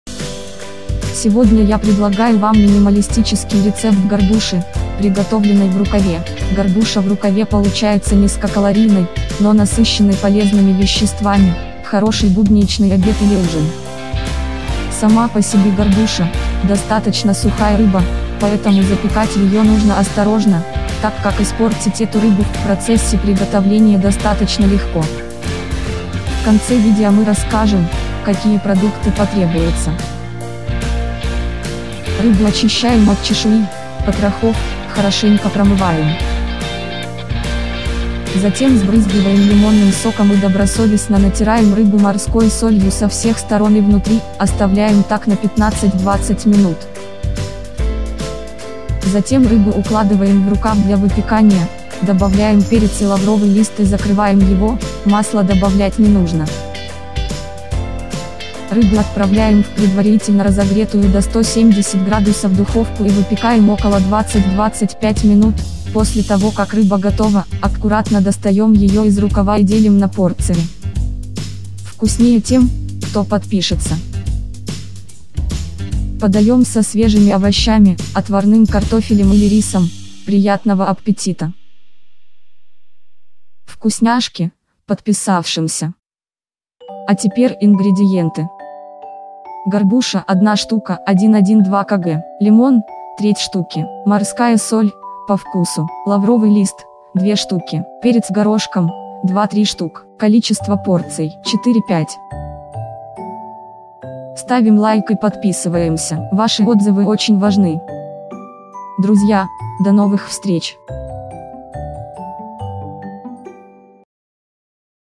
Горбуша в рукаве Рецепт, пошаговый, как приготовить, видеорецепт
Превосходный, идеальный рецепт. Сегодня я предлагаю вам минималистический рецепт горбуши, приготовленной в рукаве. Горбуша в рукаве получается низкокалорийной, но насыщенной полезными веществами. Хороший будничный обед или ужин.